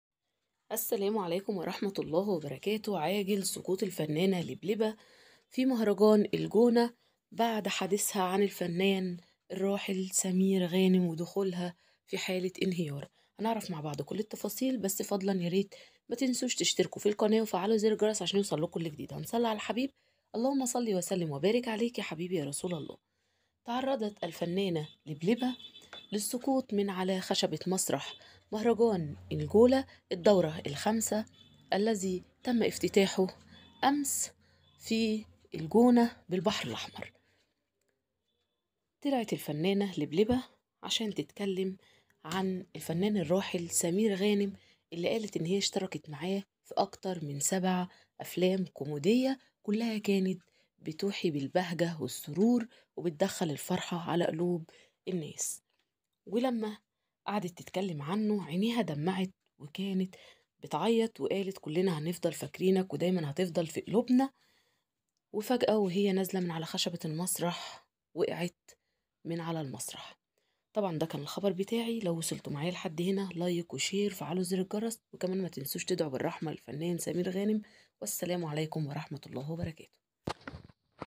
فيديو لحظة سقوط لبلبه من علي خشبة مسرح مهرجان الجونة السينمائي واصابتها💔مهرجان الجونة السينمائي
سقوط لبلبه على خشبه مسرح افتتاح مهرجان انجولا بعد حديثها عن سمير غانم
سقطت النجمه لبلبه على خشبه مسرح حفل افتتاح الدوره الخامسه من مهرجان الجونه السينمائي الذي تقدمه المذيعه ناردين فرج وذلك بعد حديثها عن النجم الراحل سمير غانم وذلك قبل عرض فيلم قصير له فيلم كوميدي ربنا يرحمه ويغفر له ويجعل مثواه الجنه

وقالت لبلبه يشرفني ان النهارده اتكلم عن عشره عمري اللي اشتغلت معي اكثر من سبع افلام وكان كلها فرحه وبهجه للناس هو صانع السعاده وعامل كوميديان خاص به هو وبس سواء مشيته اداء والتلقائي النهارده بنقول له بنحبك وهتفضل جوه قلوبنا ربنا يرحم الفنان الراحل سمير غانم
كما يتواجد في حفل الافتتاح المهندس حسام صالح المدير التنفيذي للمتحده للخدمات الاعلاميه والمهندس نجيب ساويرس، والمهندس سميح ساويرس، وبشرى زوجته، المخرج امير رمسيس، وعم رومانسي، وامل المصري المنسق العام لمهرجان الجونه، واحمد السقا وزوجته ،مها الصغيره وابنه ياسين وابنته ناديه و، يسرا و، صباح مبارك ،شيرين رضا ، نجلاء بدر وزوجها